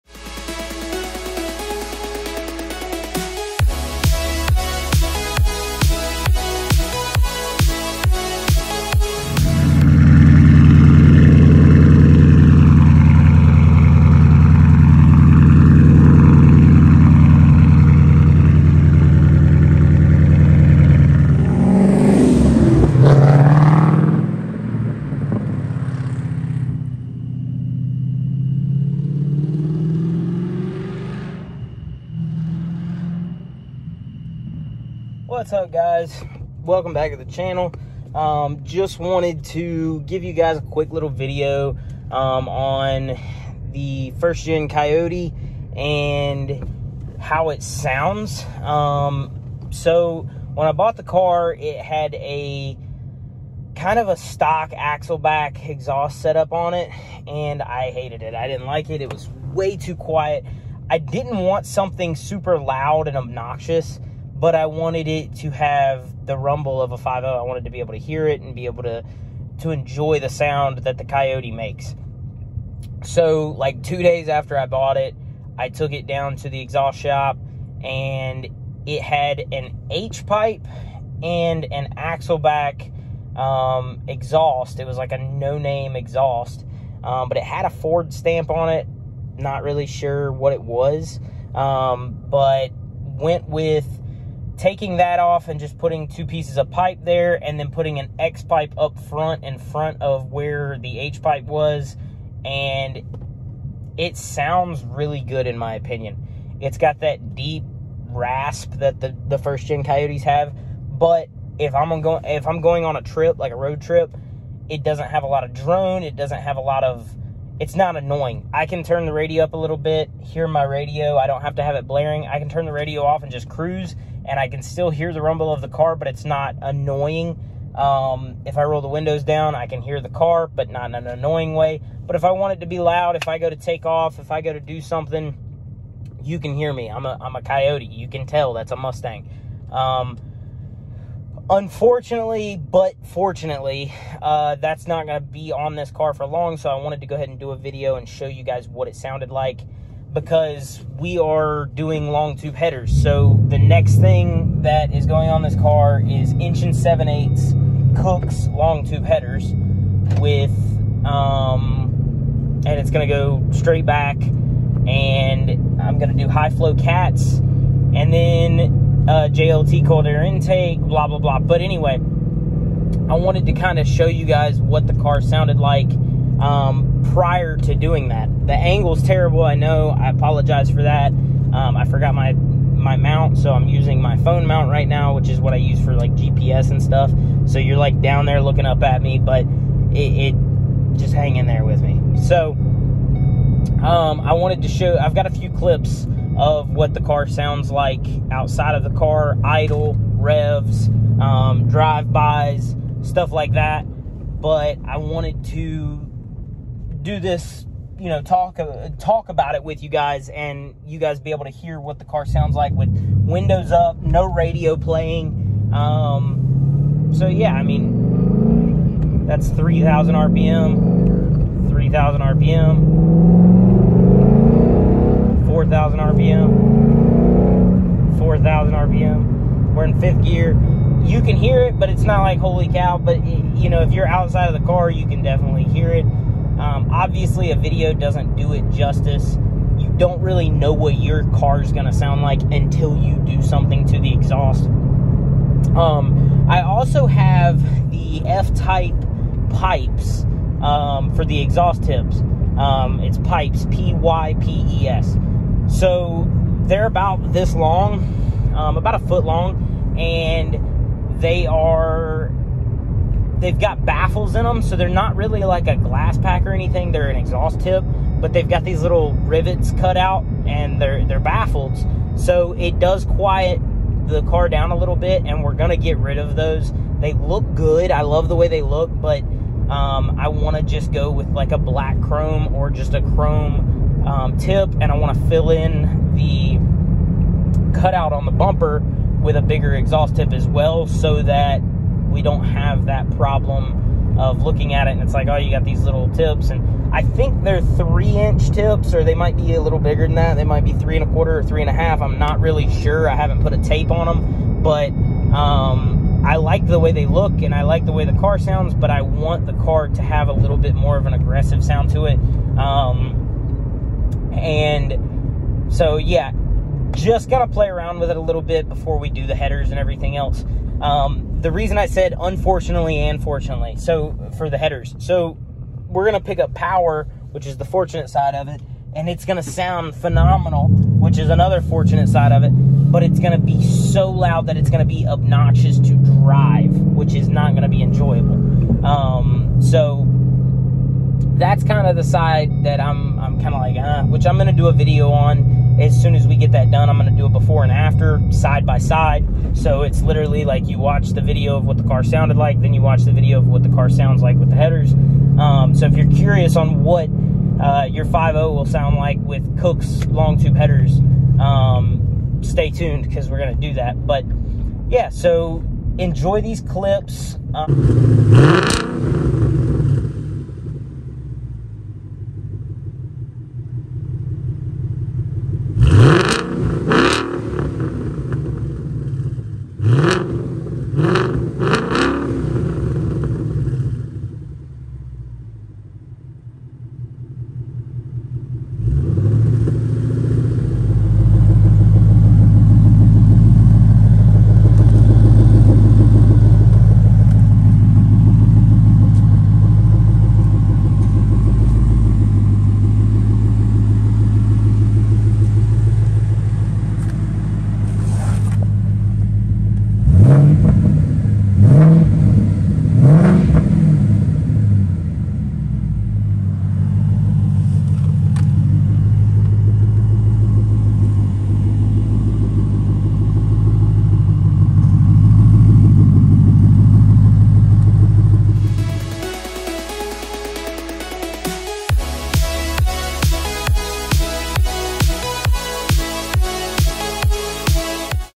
2012 mustang 1st gen coyote offroad Xpipe (no mufflers)
2012 mustang GT 5.0 coyote
offroad xpipe
BMR lowering springs
big bang 20"x9" wheels
Pypes tips
boss 302 front splitter
13-14 rear bumper conversion
RTR decklid
13-14 OEM taillights

2012 mustang 1st gen coyote offroad Xpipe (no mufflers)